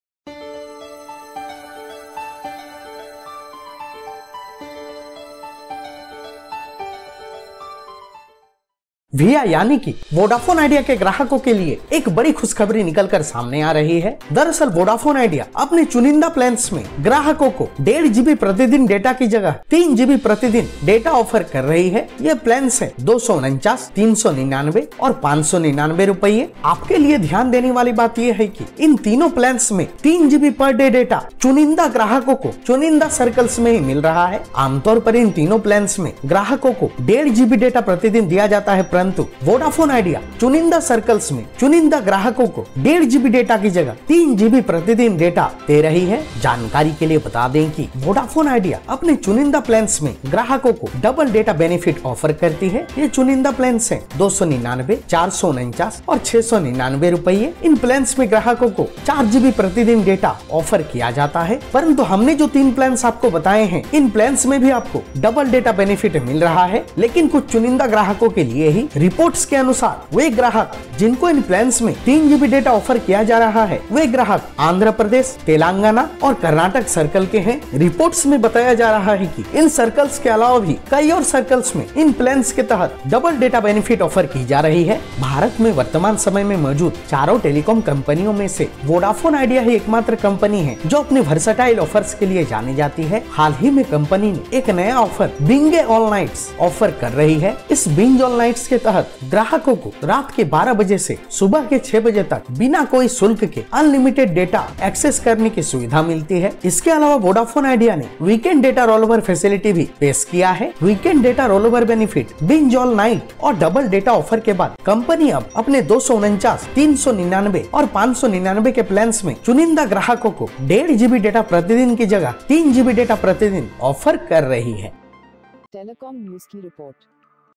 Very Good News For Vi (Vodafone Idea) Users
Very Good News For Vi (Vodafone Idea) Users.Vodafone Idea is now offering 3GB daily data with prepaid plans of Rs 249, Rs 399 and Rs 599 to select users. The three plans usually ship with 1.5GB daily data. Earlier, the same plans were part of the company’s Double Data offer, however, the offer is now valid on 2GB daily data plans of Rs 299, Rs 449 and Rs 699. The Rs 249 plan comes with 28 days validity, followed by the Rs 399 and Rs 599 plans with 56 days and 84 days validity. We have checked for the offer availability on two Vodafone Idea prepaid numbers in Hyderabad (Andhra Pradesh & Telangana circle). The offer is also available on a Vi number in Bengaluru city (Karnataka circle). So it seems like Vi is providing the same offer in several circles, although only to select users.

Vodafone Idea is the only telecom operator with the most versatile prepaid plans and offers right now. Last week, the struggling telecom operator introduced a new scheme called ‘Binge All Night‘ as part of which customers can enjoy unlimited data without any limit between 12 AM and 6 AM. Besides, Vi also has the Weekend Data Rollover facility.

The Rs 249, Rs 399 and Rs 599 prepaid plans are shipping with 3GB data per day for select users (up from the regular 1.5GB data per day benefit). The plans come with 28 days, 56 days and 84 days validity. Combined with the weekend data rollover facility, the three plans truly value for money. Vi’s Double Data offer continues to be available on Rs 299, Rs 449 and Rs 699 prepaid plans as they come with 4GB data per day.

Other benefits of all the six plans mentioned above include unlimited voice calls to any network within India without any FUP limit, 100 SMSes per day and Vi Movies & TV subscription. Did you receive this offer from Vi? Let us know by commenting below.

In other news, the latest Trai monthly subscription report for December 2020 showed Vi lost more than 5.5 million subscribers in a single month. The telco ended Q3 FY21 with 269.8 million subscribers by losing just two million subscribers during the entire quarter. With such customer-centric moves, Vodafone Idea slowly gaining back momentum, but the telco’ struggles are far from over. Furthermore, Vi’s network is slowly becoming better across the country as it finishes network integration and also started the refarming 3G spectrum for 4G. Vi is expected to phase out 3G entirely by the end of FY22.